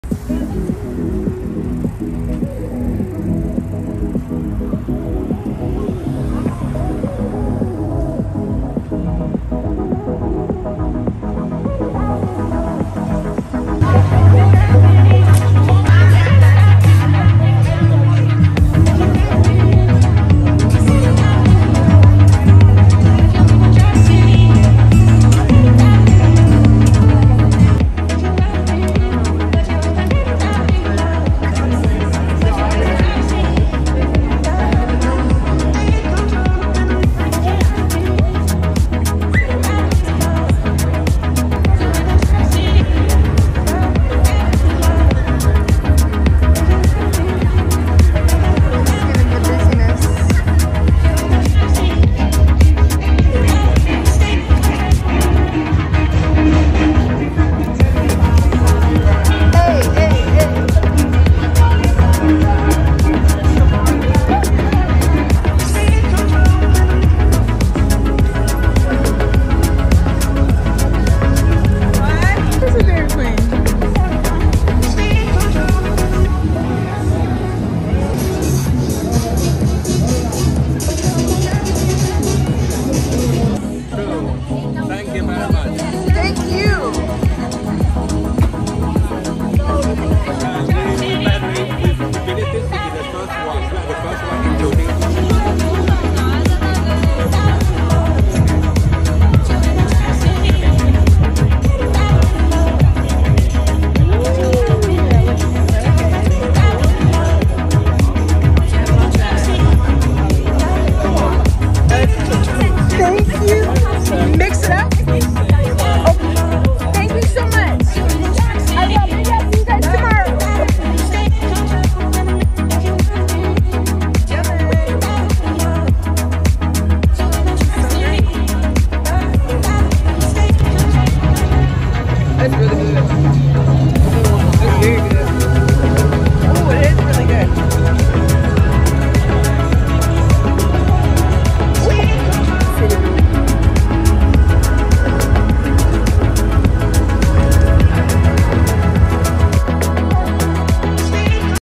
Toronto Chinatown Festival | TRAVEL VLOG | AUGUST 2023
We stumbled across Toronto’s  Chinatown Festival.

I don’t get a chance to go to Toronto to often, so I was not aware they hold a Chinatown festival annually. Now that I am in the know I look forward to going back and making a weekend out of it.